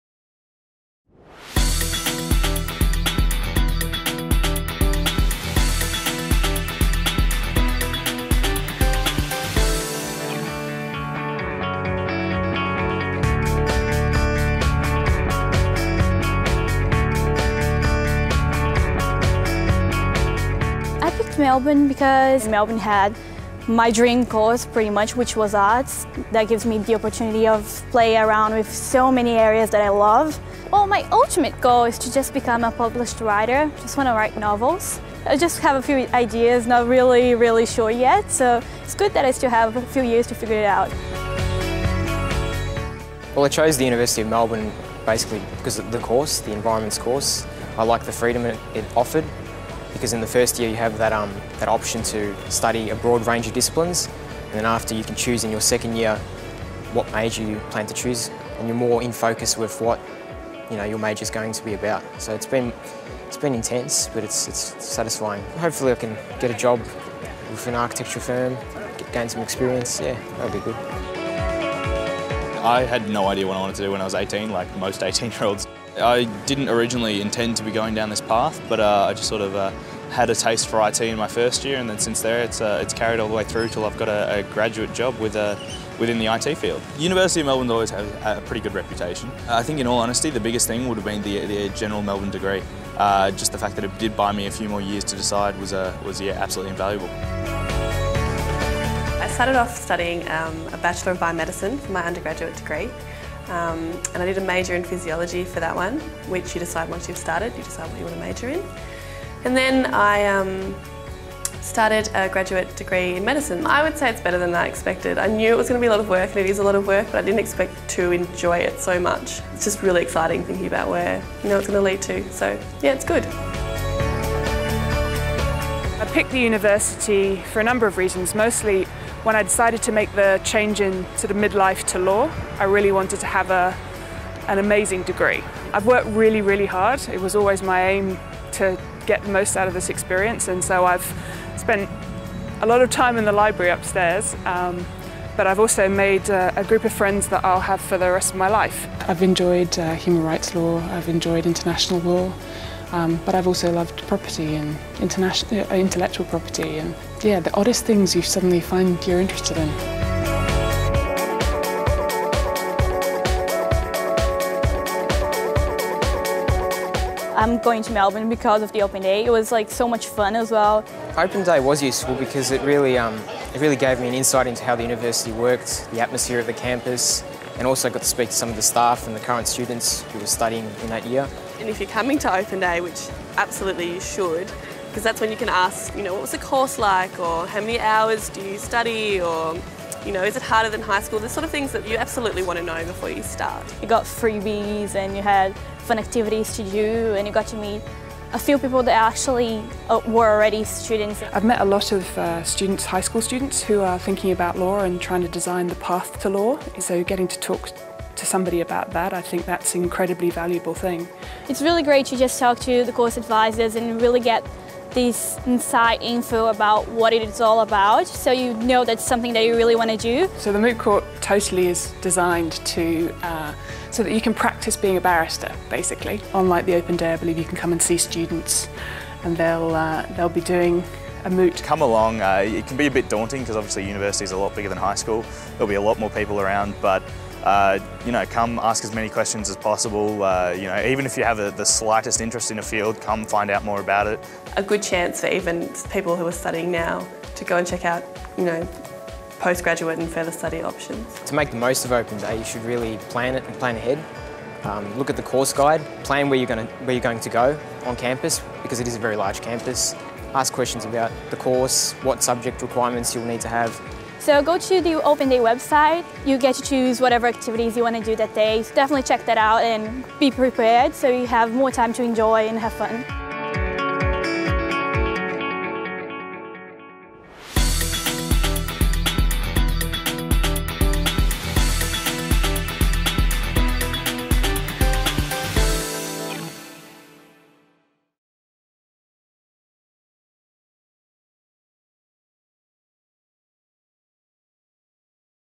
Visions: Open Day Preview 2011
The flags have gone up. The brochures have been printed.  With only a few days to go until Open Day, Visions has a preview of what it's like to be a University of Melbourne student at all stages of their educational journey, and what they can expect on the day.

Plan your day and get all the information you need for Open Day

Credits

Produced by Lyn Toh
Edited by Robert Cross
Camera by Clive Banfield
Open Day 2010 footage provided courtesy of Ben Loveridge and Digital Media Solutions, University of Melbourne.

With thanks to Ben Loveridge, Kathryn Boin, Ron Baird and Taryn Groom.